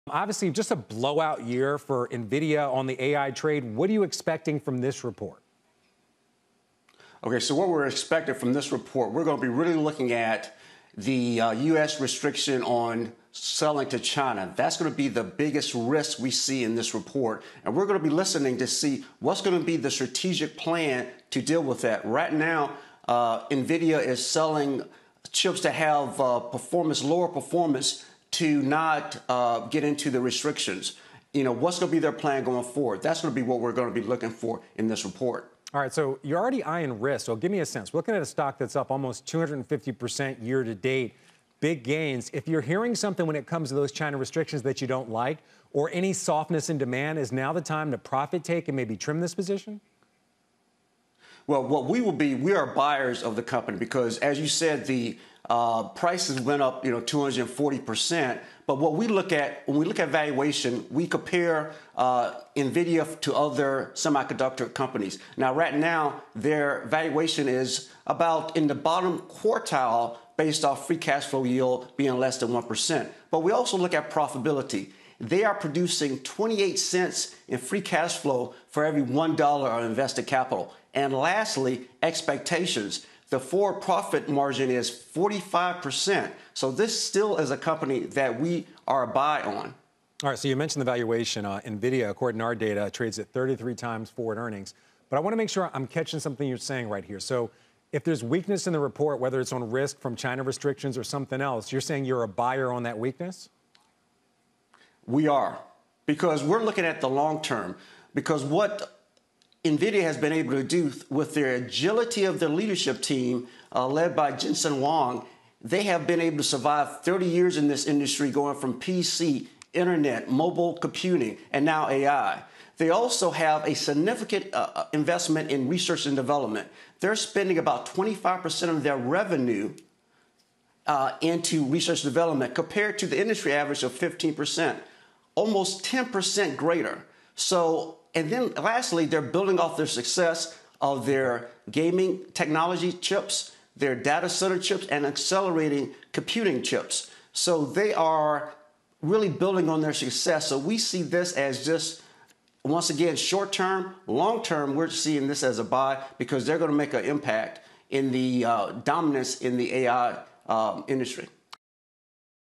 The major risk to Nvidia earnings is its relationship with China, says Degas Wright
Degas Wright, Founder, CEO, and CIO of Decatur Capital, discusses his expectations for Nvidia.